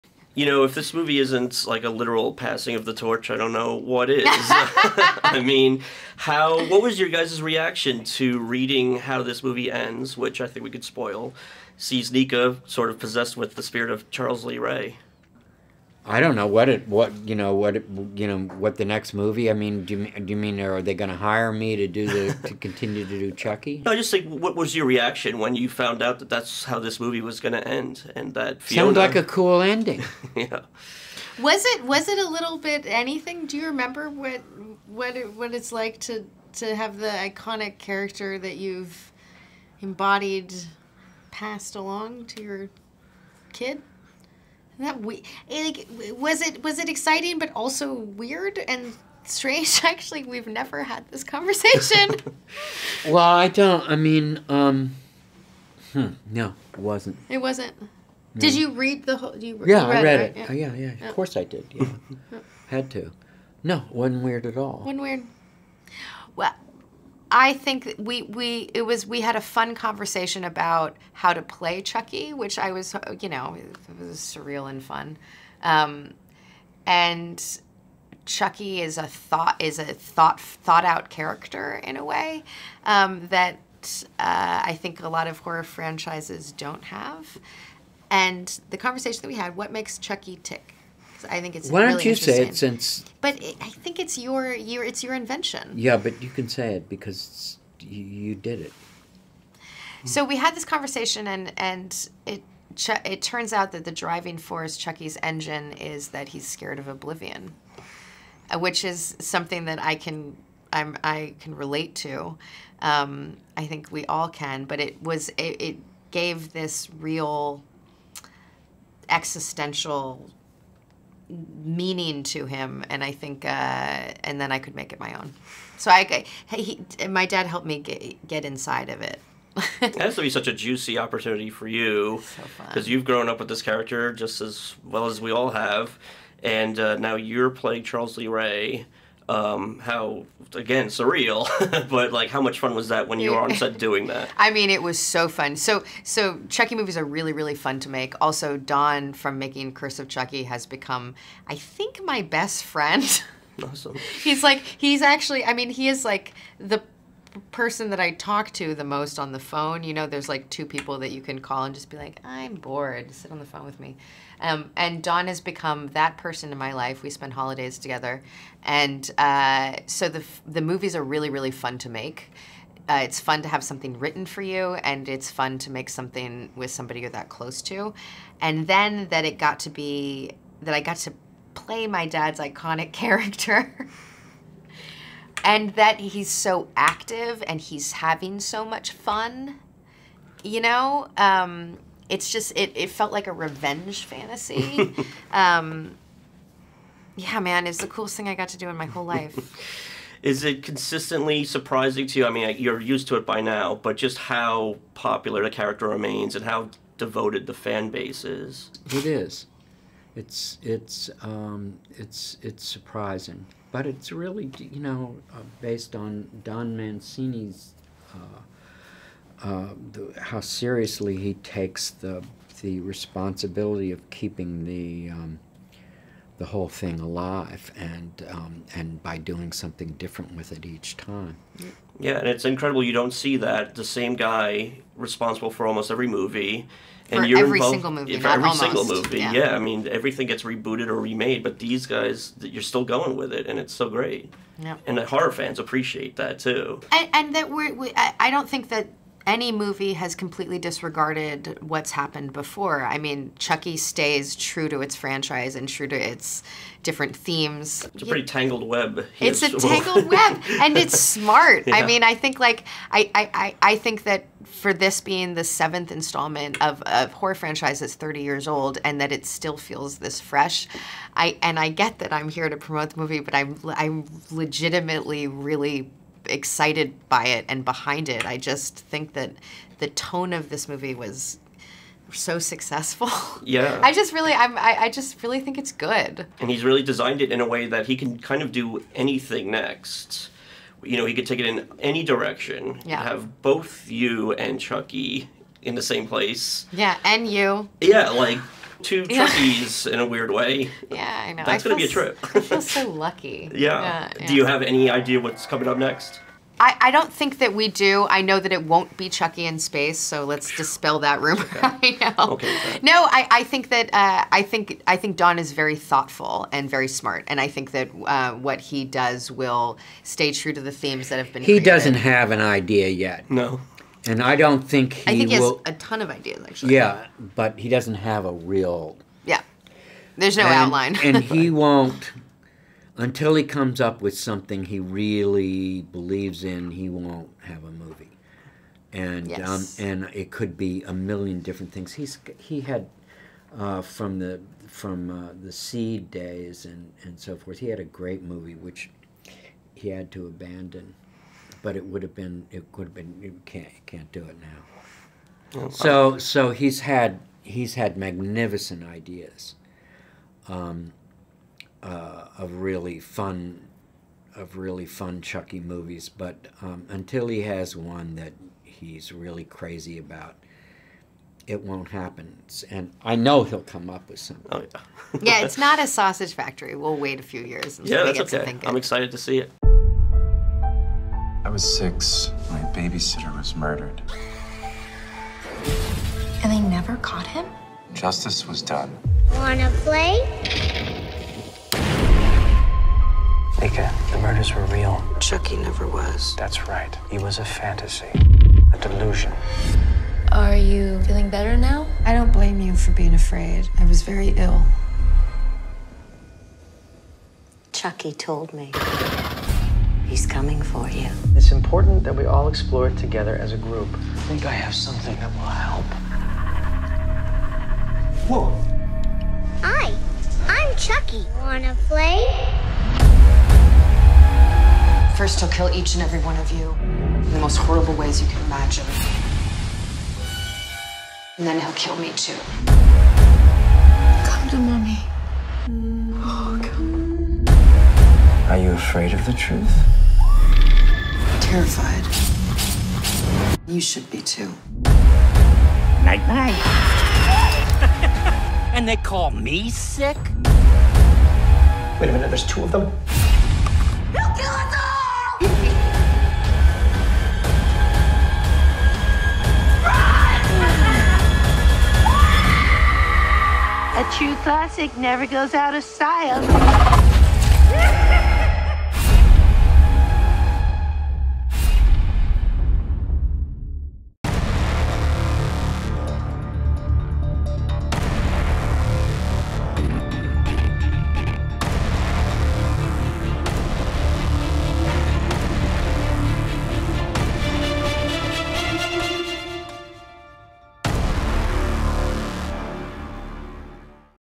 Fiona Dourif and Brad Dourif interview - CULT OF CHUCKY (2017) horror movie
PLOT: Chucky returns to terrorize his human victim, Nica. Meanwhile, the killer doll has some scores to settle with his old enemies, with the help of his former wife.